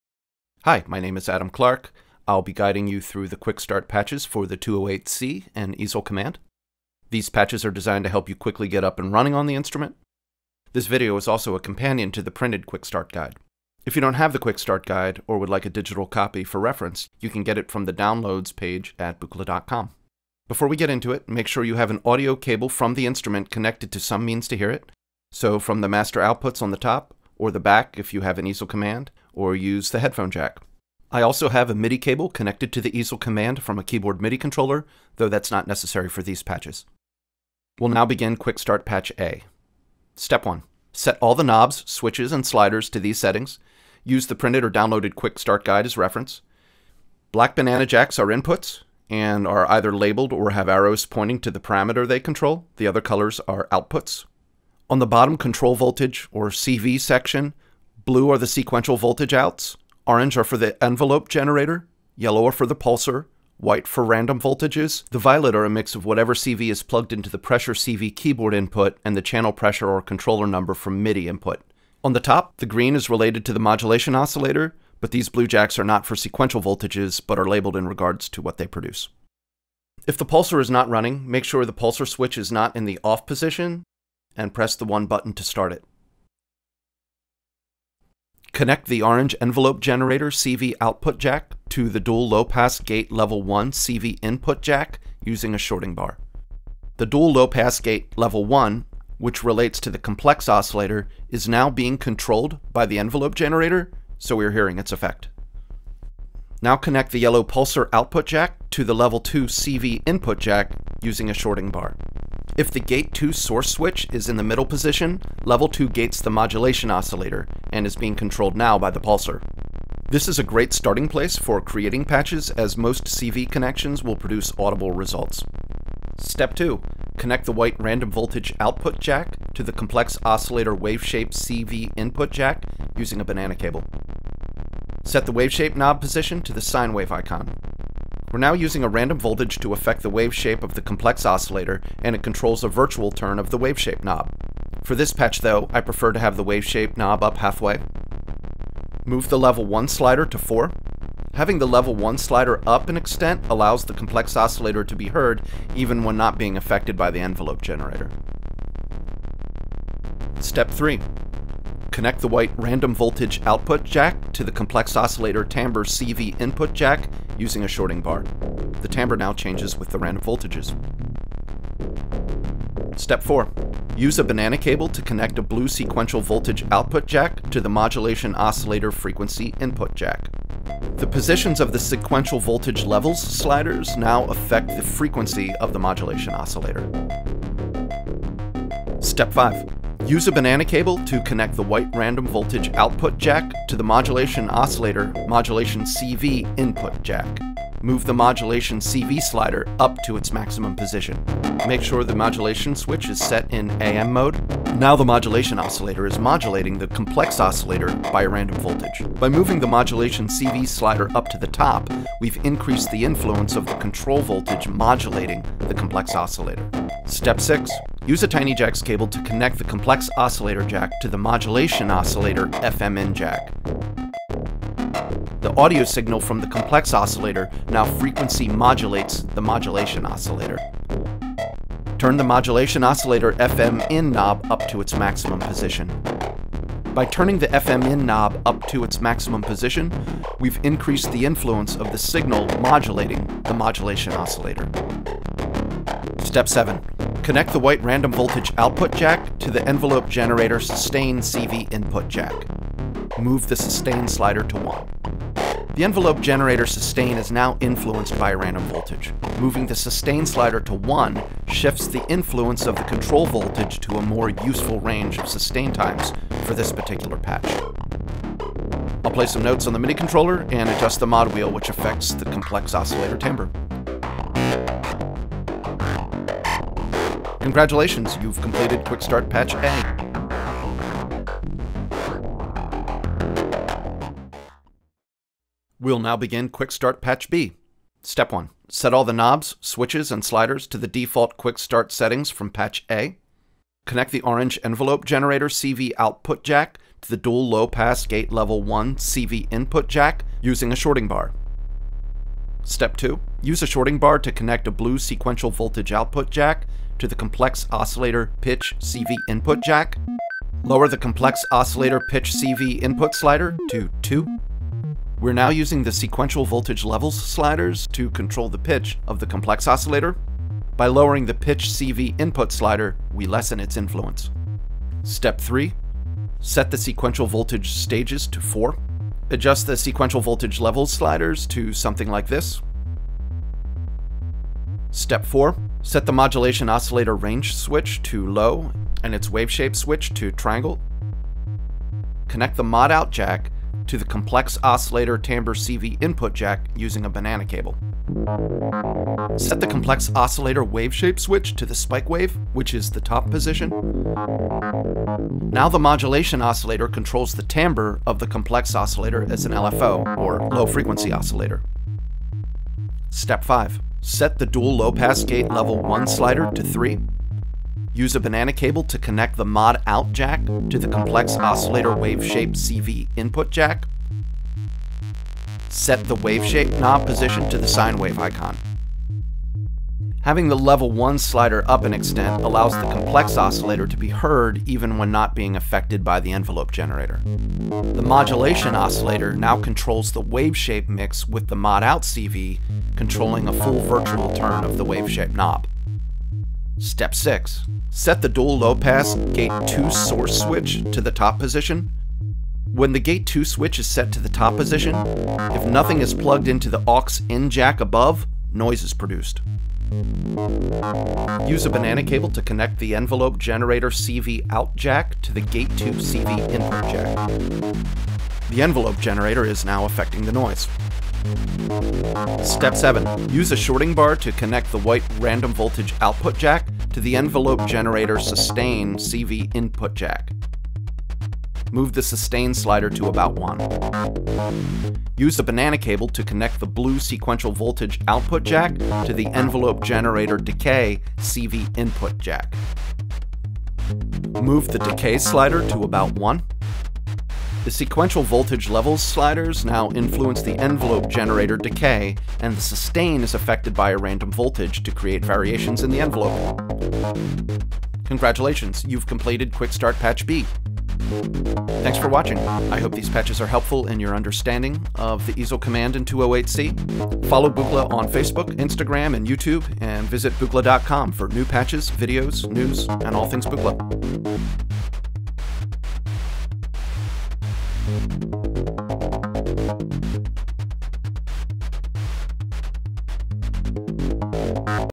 Easel Command & 208C Quickstart Guide Video
The video counterpart to the printed quick start guide included with Buchla Easel Command & 208C. Download a digital copy of the quickstart guide